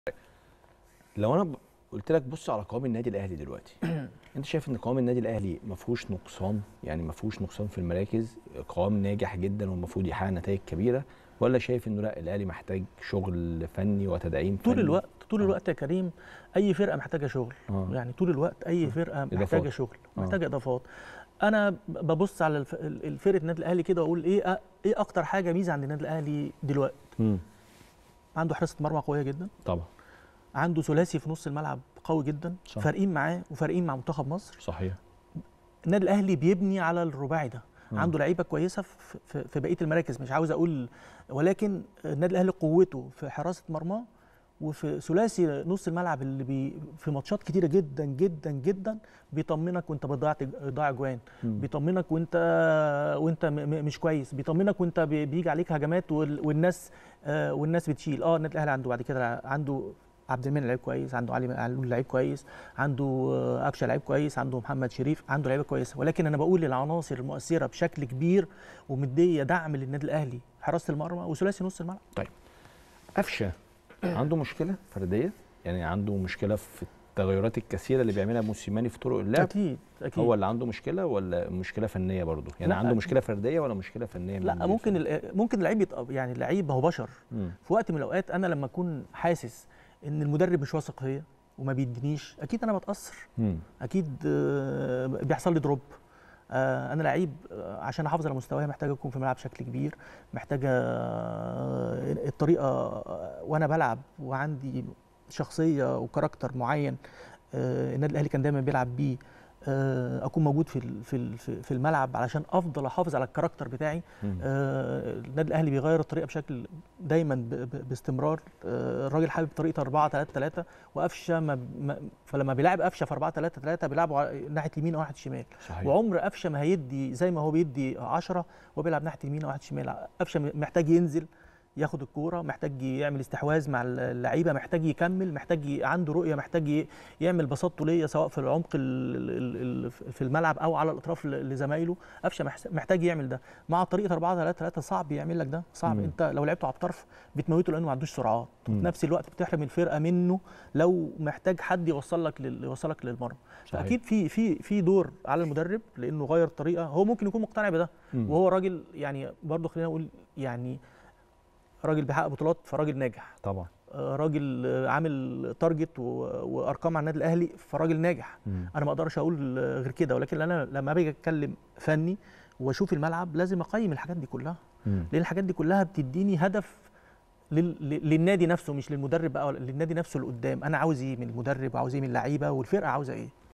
أحمد أيوب: قوة الأهلي في حراسة المرمى وخط الوسط اللي فرق مع منتخب مصر وهناك قصور في هذه المراكز
رقم 10 | أحمد أيوب: قوة الأهلي في حراسة المرمى وخط الوسط اللي فرق مع منتخب مصر وهناك قصور في هذه المراكز